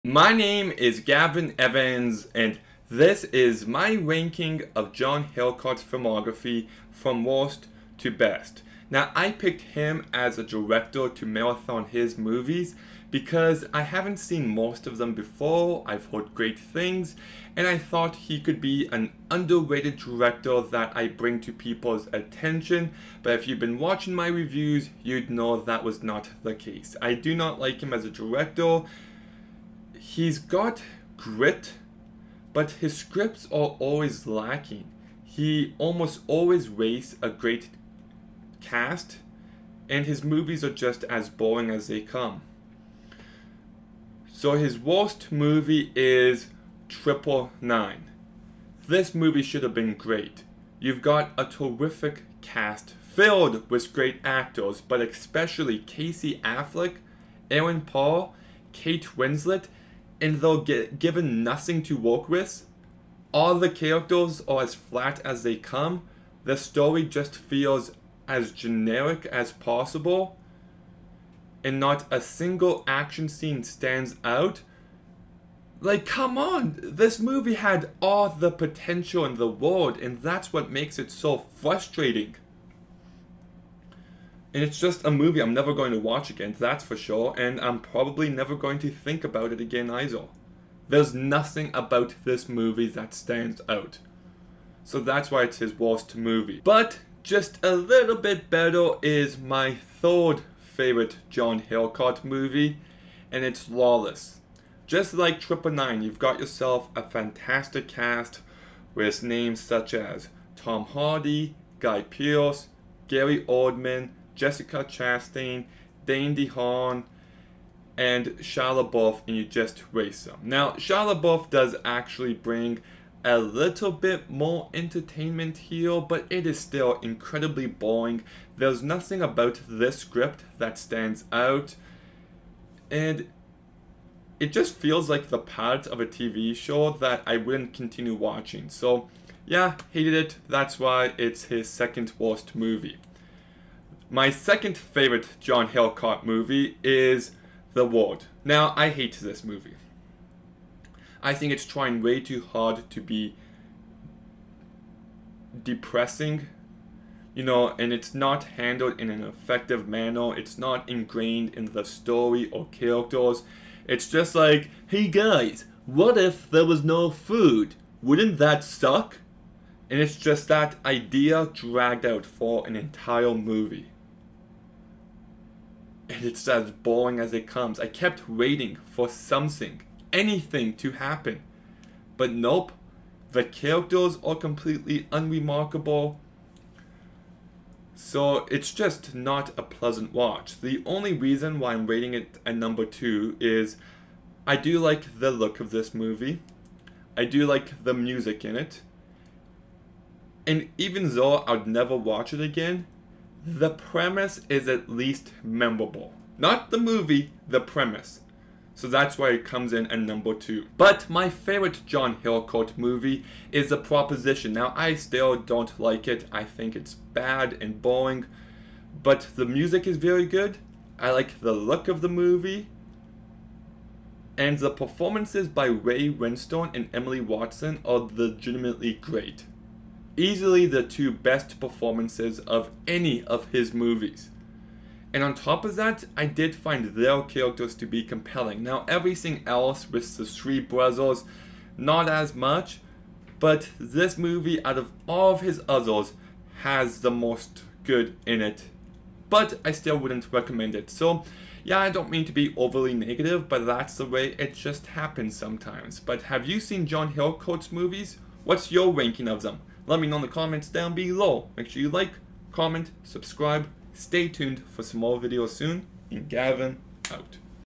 John Hillcoat Movies Ranked
Here's my ranking of John Hillcoat's filmography from worst to best.